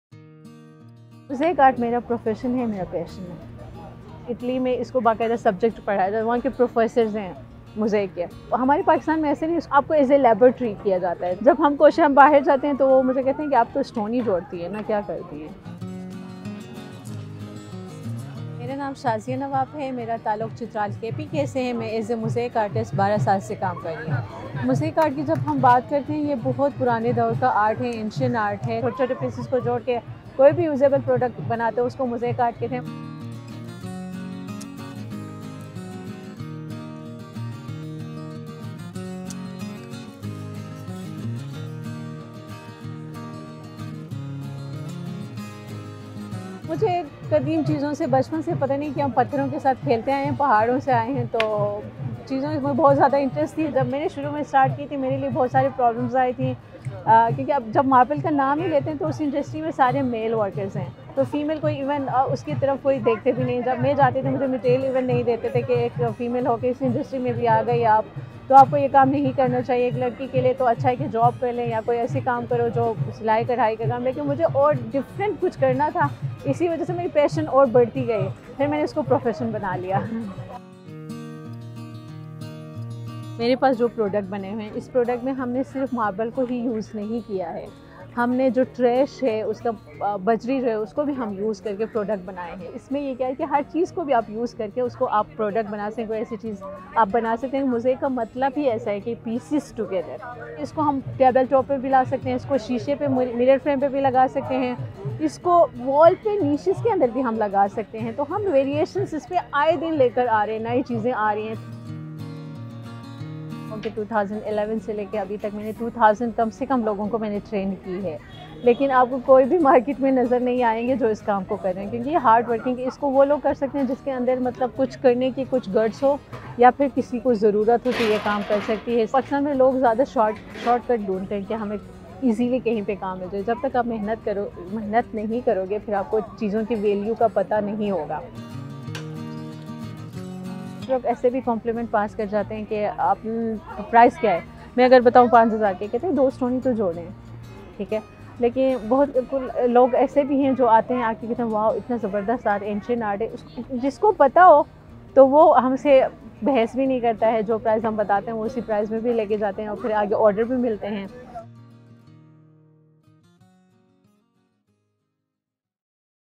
Meet a Chitrali mosaic artist | VOA URDU
ماربل کے چھوٹے چھوٹے ٹکڑے جوڑ کر مختلف فن پارے بنانا موزیک آرٹ کہلاتا ہے۔ پاکستان میں اس قدیم آرٹ سے کئی لوگ وابستہ ہیں۔ وائس آف امریکہ کے نذر الاسلام ایک ایسی ہی خاتون آرٹسٹ سے ملا رہے ہیں جنہوں نے موزیک آرٹ کو بطور پیشہ اپنایا ہے۔